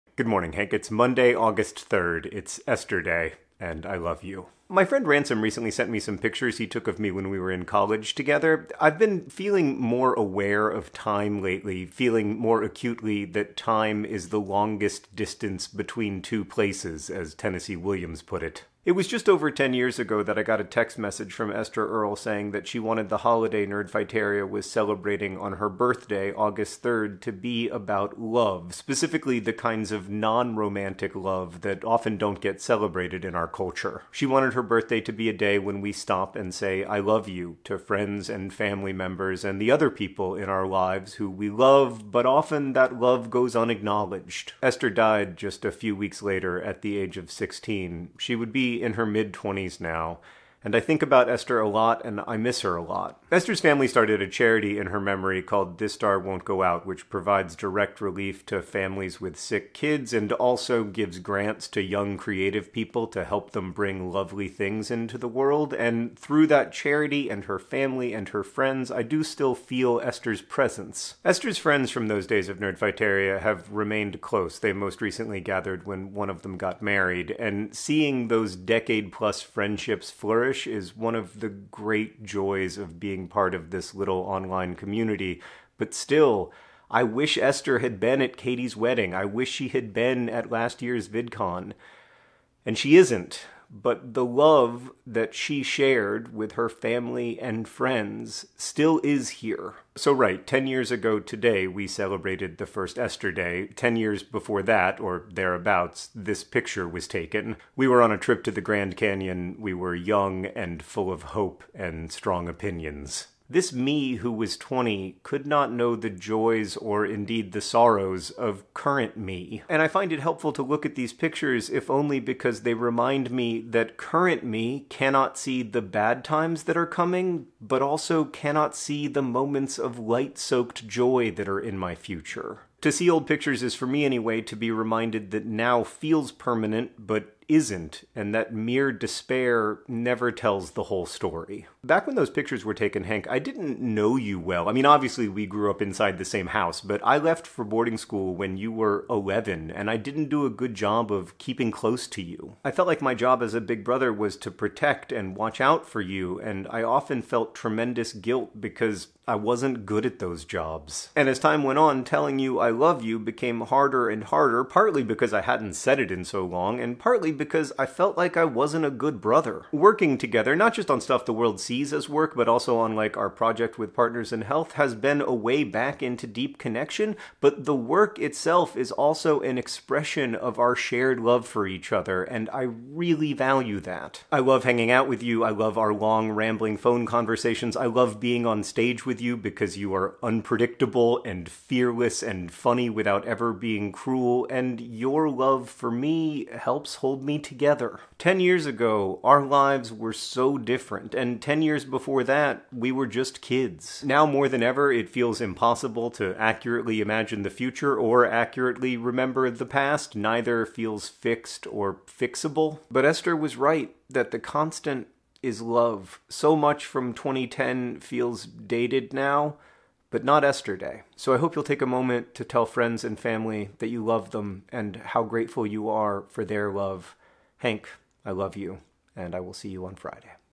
you're bundled up now? wait till you get older.
In which John celebrates Esther Day 2020 by talking about old pictures, love, and Esther Earl.

If you're able to donate $2,000 or more to this effort, please join our matching fund